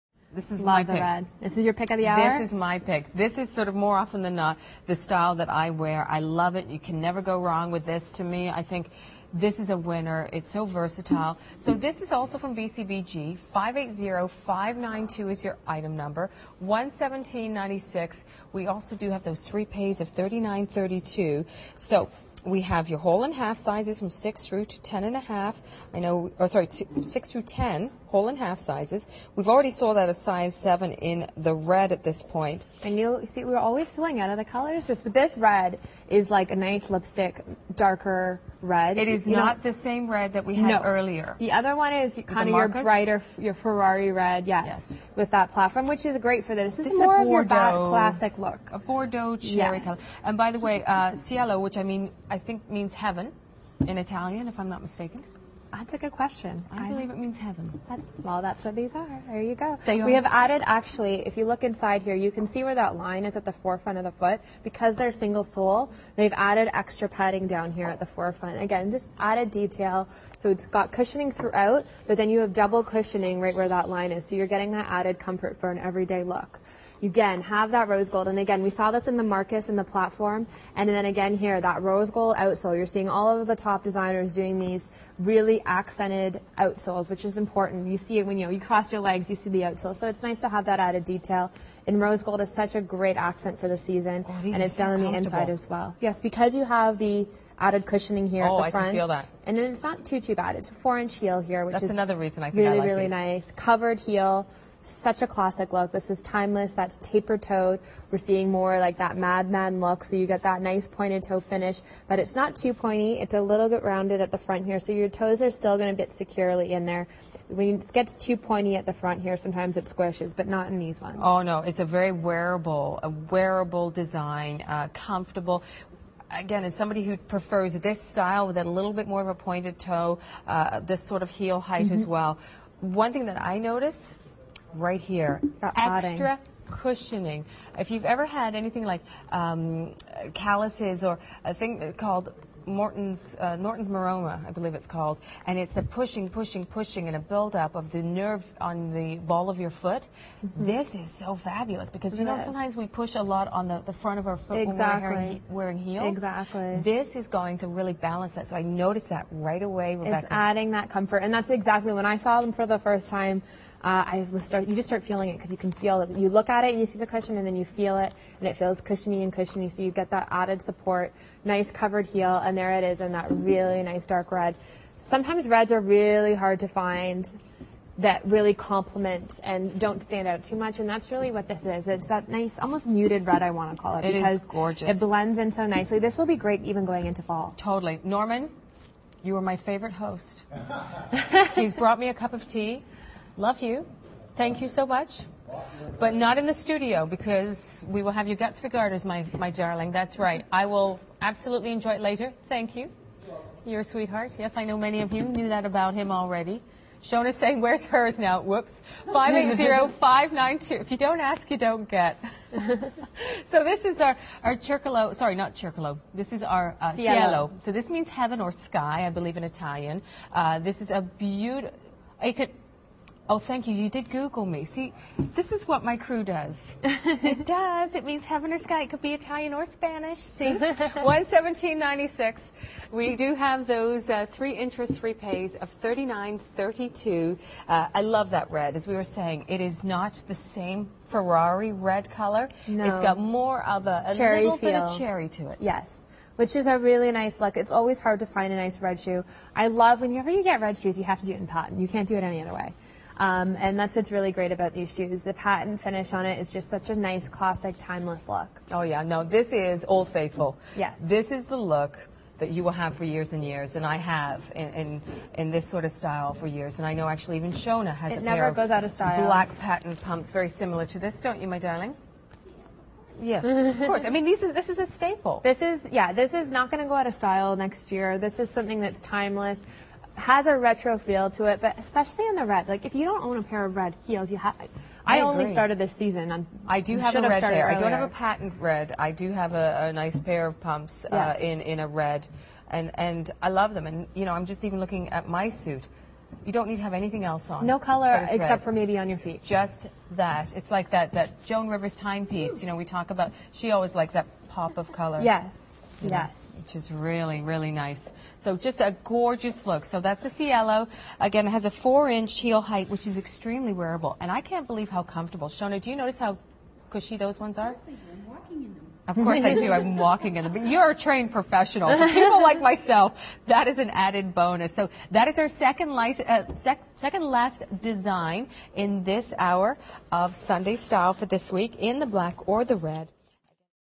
BCBGeneration Cielo Pump at The Shopping Channel 580592
There is no such thing as a woman having too many pairs of shoes. Especially if the next pair you get are as spectacular as these Cielo pumps from the BCBGeneration Shoe Collection. Featuring a classy closed pointed toe and a lightly padded insole, they are the prefect combination of style and wearability.

• Fabric: Man made materials
• 4" heel height
• Regular width
• Care: Wipe with damp cloth